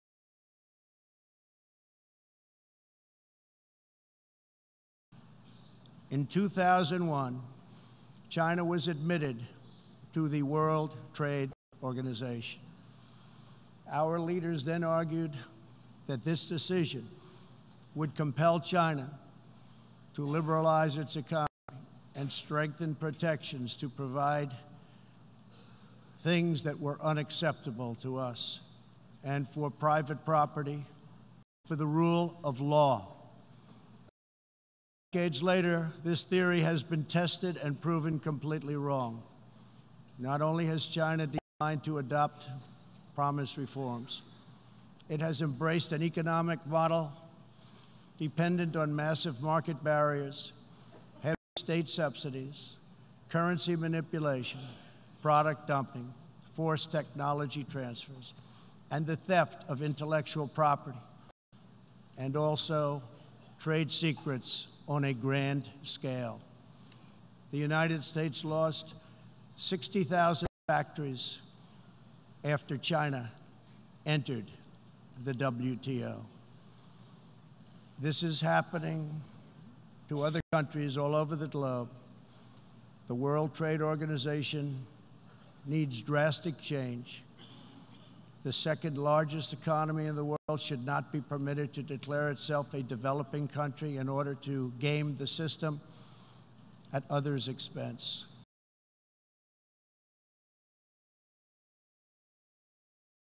Prez Trump slams China at UNGA over its trade policies
New York (US), Sep 24 (ANI): Unites States President Donald Trump addressed at United Nations General Assembly in New York. Speaking on US-China relationship, Trump said, “In 2001 China was admitted to the World Trade Org. Our leaders then argued that this decision would compel China to liberalise its economy and strengthen protections to provide things that were unacceptable to us. And for private property for the rule of law. Decades later this theory has been proven wrong. Not only has China declined to adopt promised reforms, it has embraced an economic model dependent on massive market barriers, currency manipulation, product dumping, forced technology transfers and theft of intellectual property.”
Further attacking China over its trade policies, Trump added, “US lost 60,000 factories after China entered WTO. This is happening to other countries all over the globe. WTO needs drastic change. 2nd largest economy in world shouldn't be allowed to declare itself a developing country in order to game the system at others' expense.”